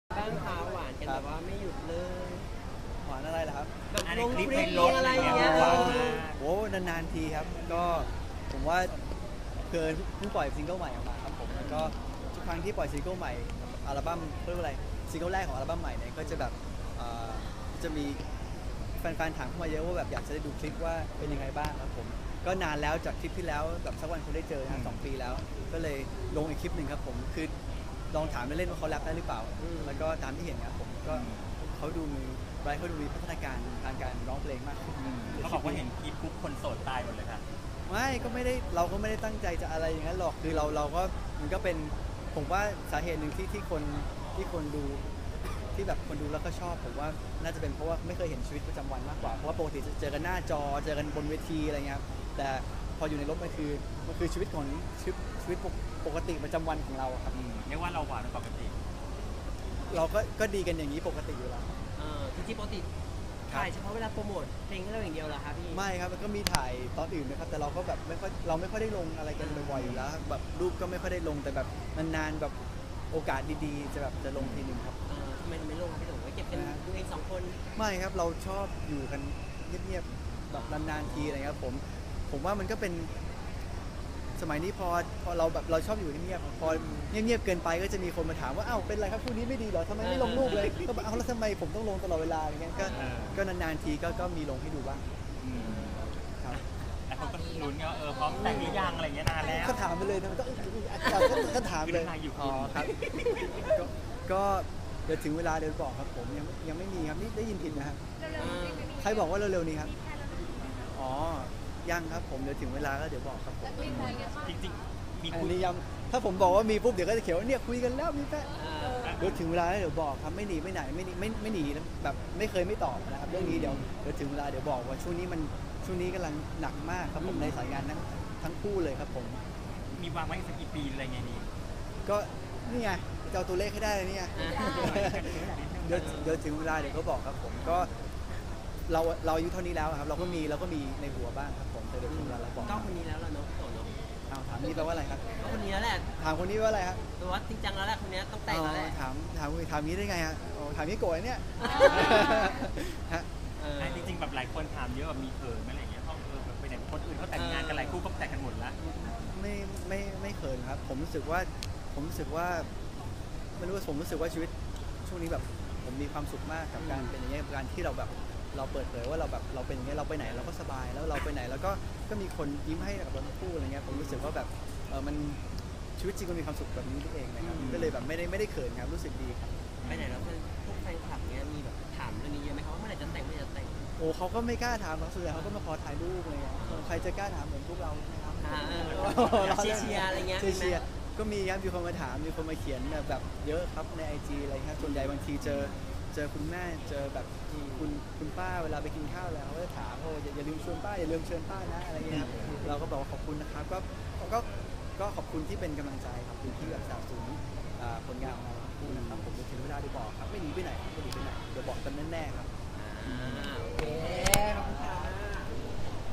“โต๋” โต้เตรียมแต่ง “ไบรท์” เปิดใจคลิปหวานฆ่าคนโสด แค่โปรโมตซิงเกิลใหม่
“โต๋” โต้เตรียมแต่ง “ไบรท์” เปิดใจคลิปหวานฆ่าคนโสด แค่โปรโมตซิงเกิลใหม่ 12/11/2018